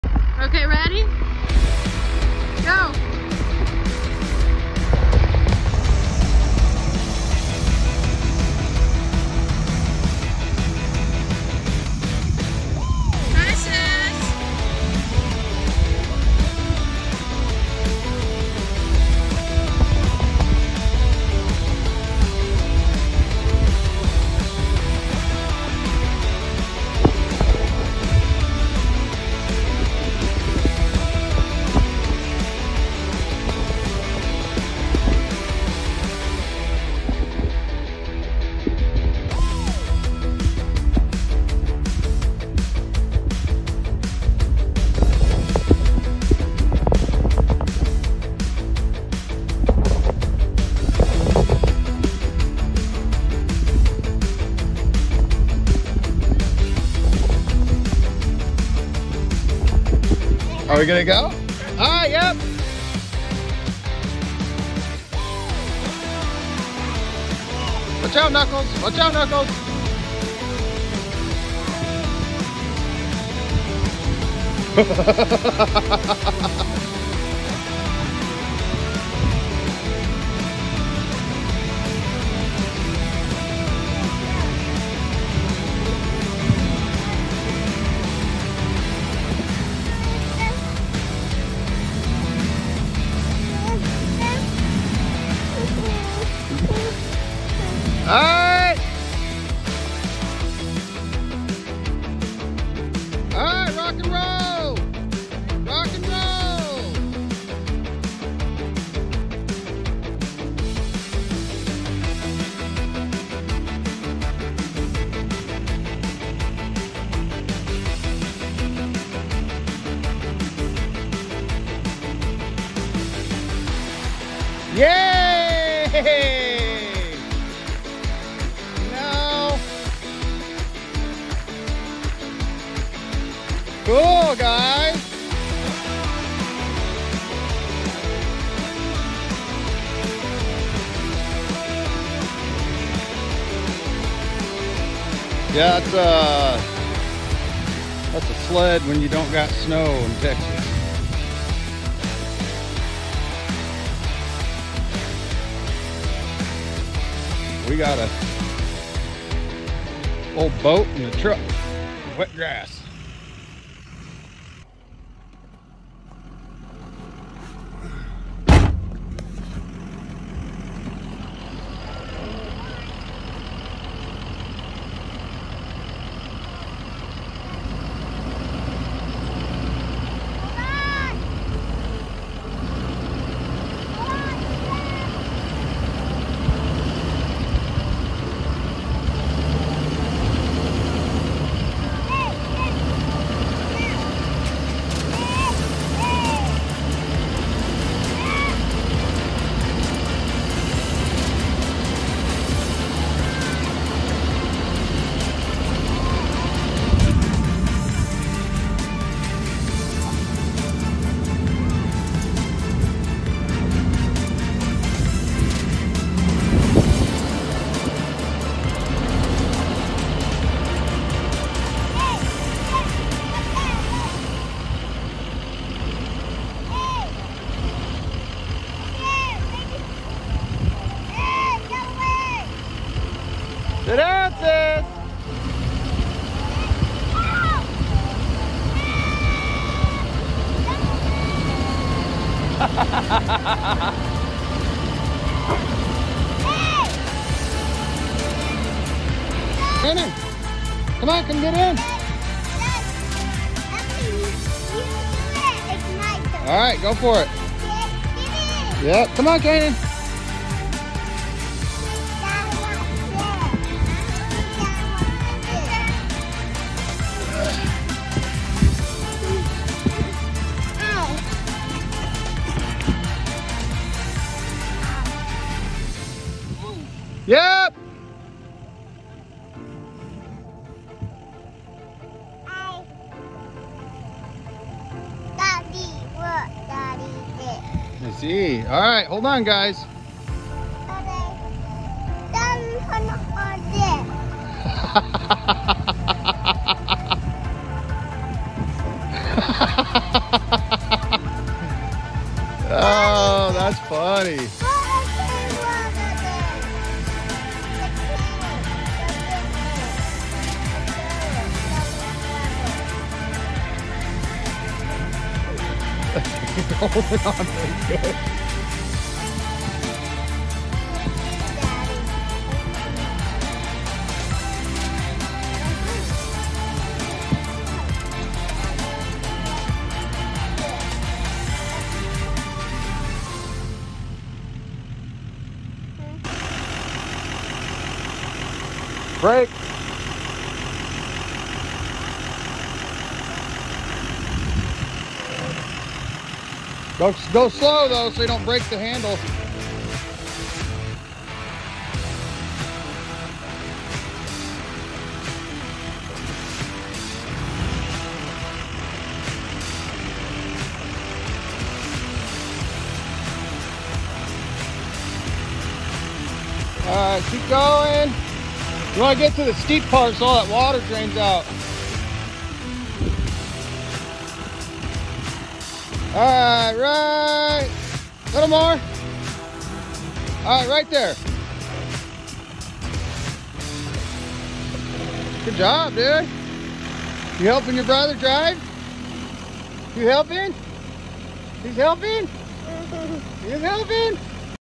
Using An Old Boat As A Sled On Grass! I Knew That Old Boat Was Good For Something!
Using An Old Boat As A Sled On Grass! I Knew That Old Boat Was Good For Something! My Son Did Most Of The Driving, He’s 6!

Towing, how to tow a boat, redneck